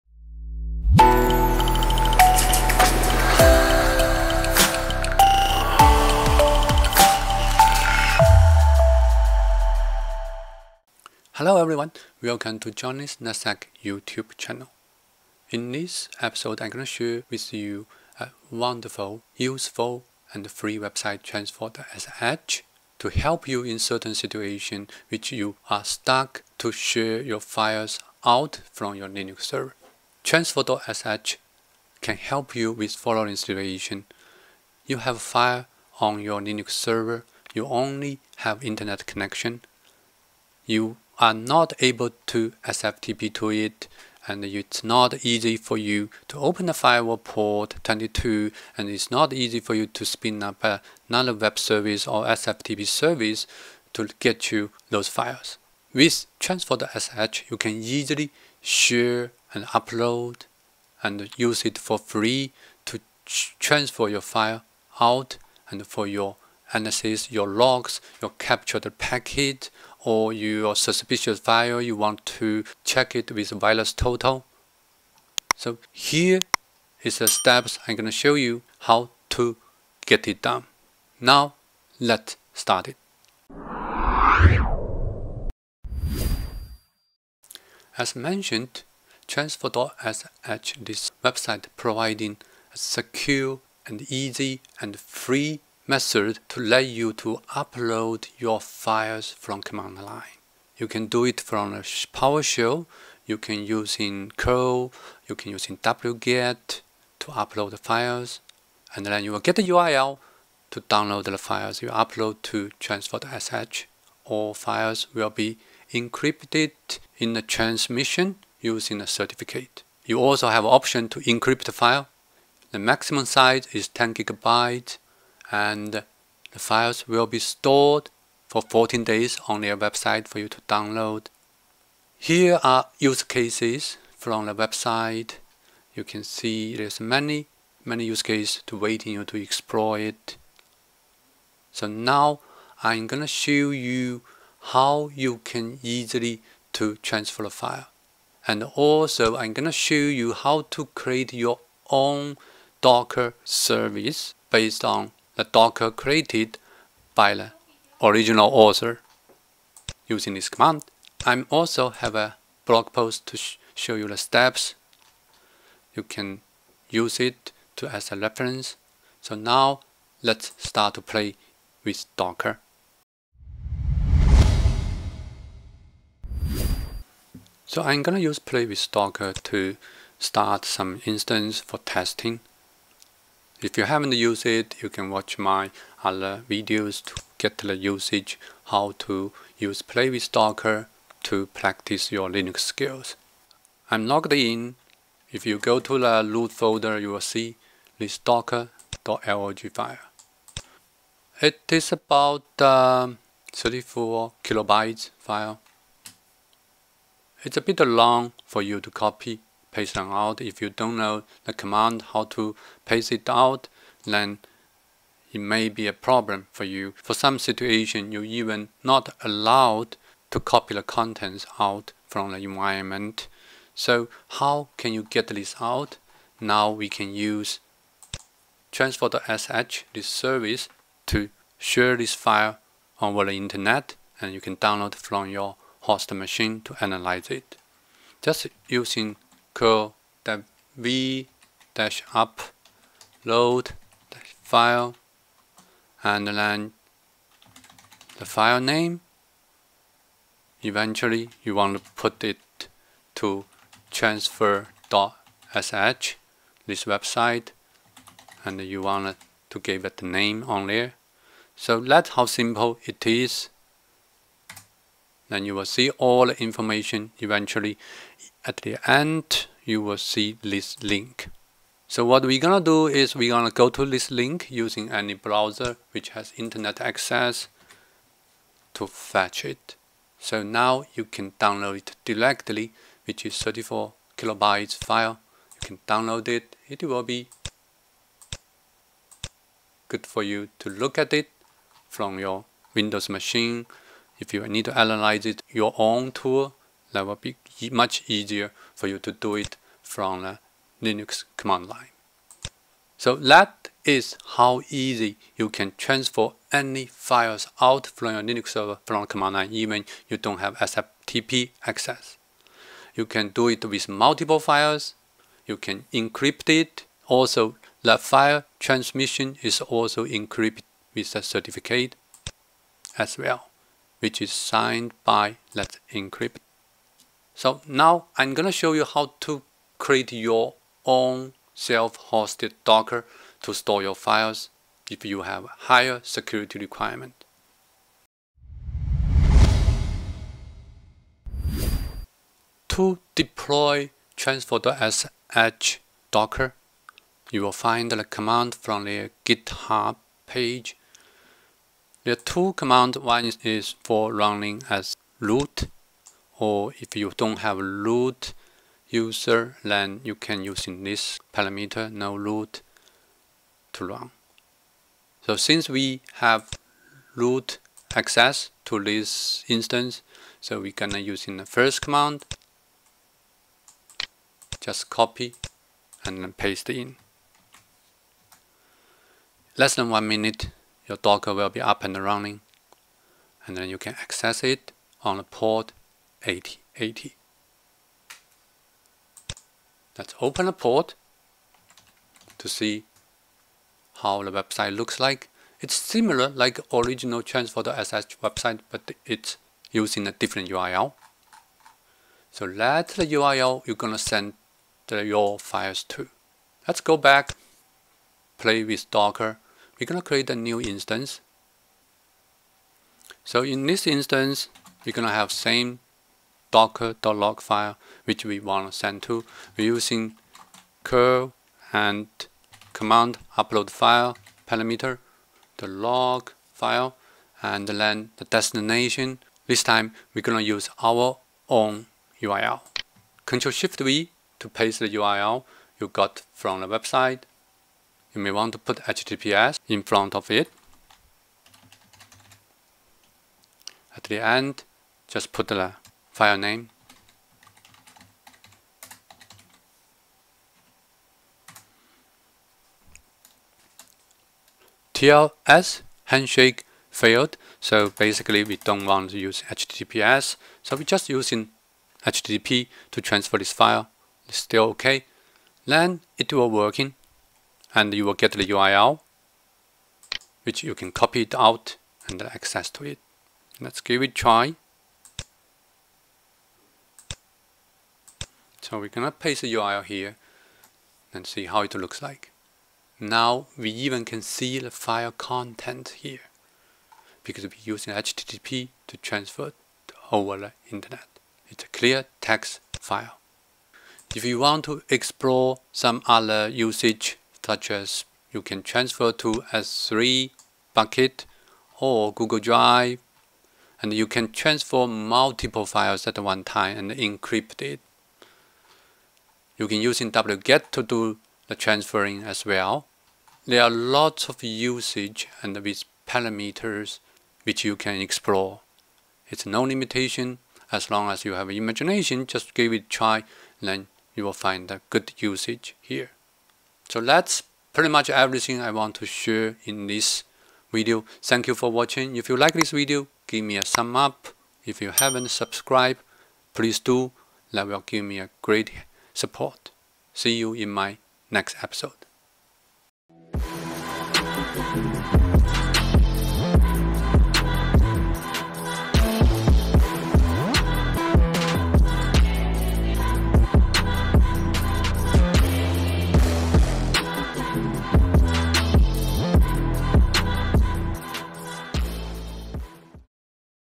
[5 Mins Docker] Secure and Easy To Transfer Files Out From Command Line - Transfer.sh
This video shows you an online service from transfer.sh which allows you to use command line to transfer your file out securely and easily. It is completely free. File size is 10GB maximum and only stores for 14 days, but it will be more than enough in normal situations.

We also can deploy a docker to have your own transfer.sh service for your own environment.

🌟One Command to Lauch Linux VNC Desktop Docker From Free 16G Ram 4vCPU Linux Root Access GCP Shell

💖Chapters:
0:00 - Introduction
1:24 - Lets Start it
2:52 - 1. Upload One File to Transfer.sh
5:38 - 2. Create Your Own Docker for File Transferring
9:02 -  End scene

Learning and Sharing - 🔊海内存知己,天涯若比邻!